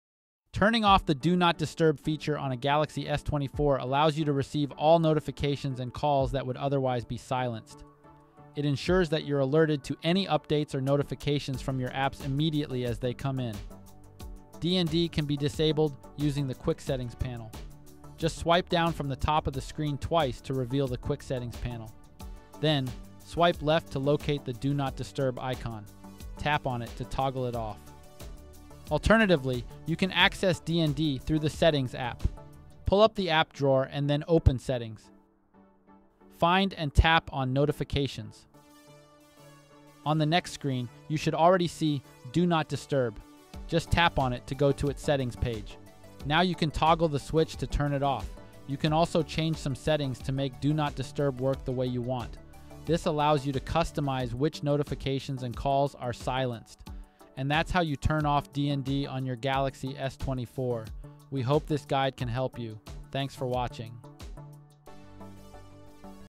How To Turn Off Do Not Disturb On Samsung Galaxy S24
In this video, you'll learn how to disable the Do Not Disturb feature on your Galaxy s24, ensuring you don't miss any notifications or calls that would otherwise be silenced. Simply swipe down from the top of your screen twice to access the Quick Settings panel, locate the Do Not Disturb icon, and tap to toggle it off for immediate access to all updates. Alternatively, you can navigate through the settings app to customize your Do Not Disturb preferences, allowing you to tailor which notifications and calls are silenced to suit your needs.